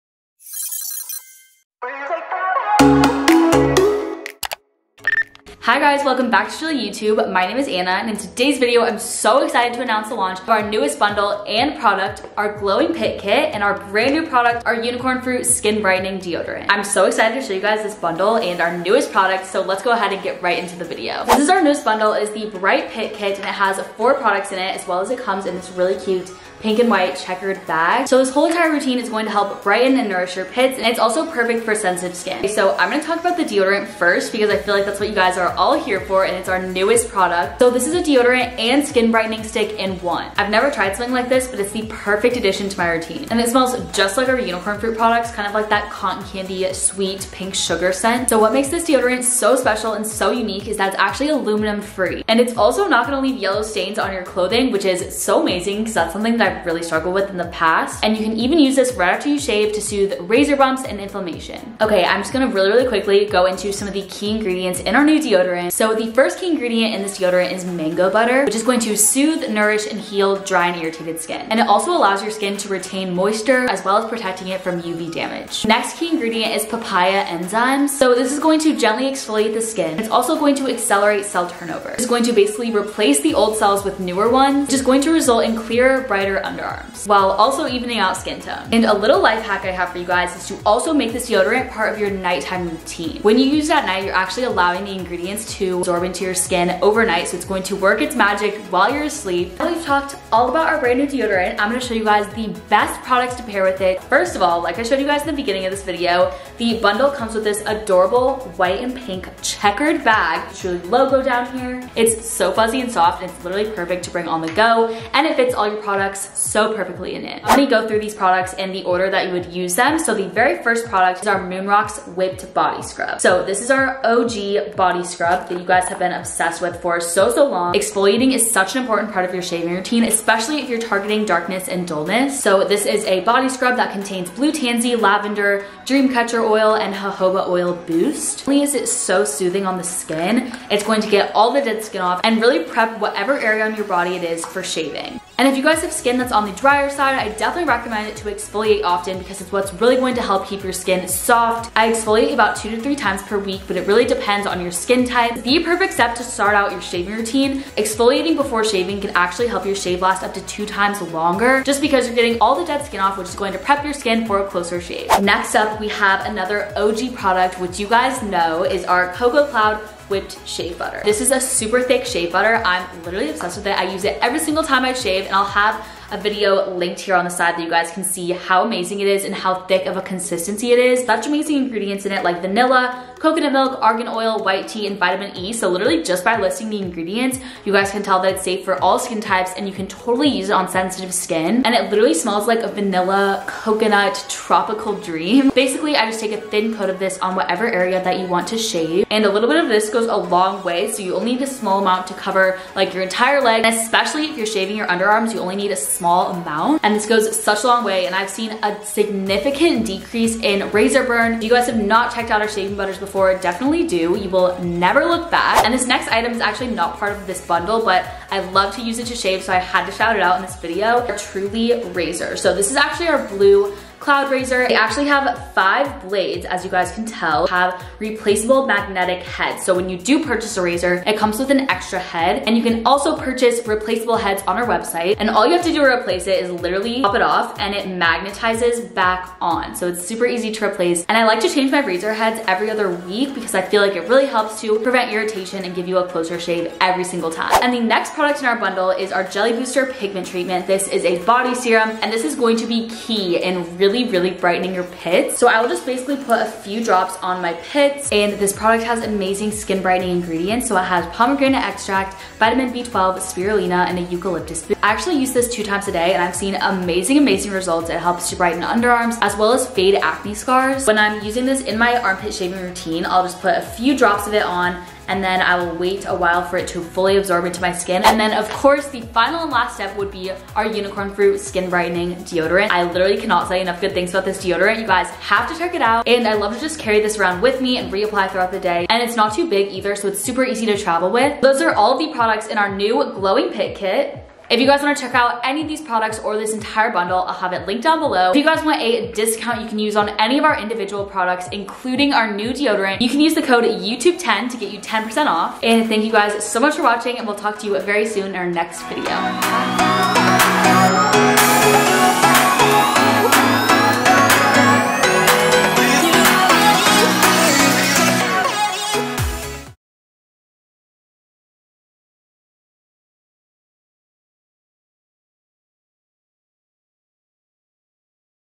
The Truth About How I Brightened My Dark Armpits INSTANT RESULTS | Truly Beauty
In today's video, Anna is showing off our NEW Pit Perfecting Kit including our new Unicorn Fruit Deodorant!

Comment if you enjoyed and which product you're MOST excited about!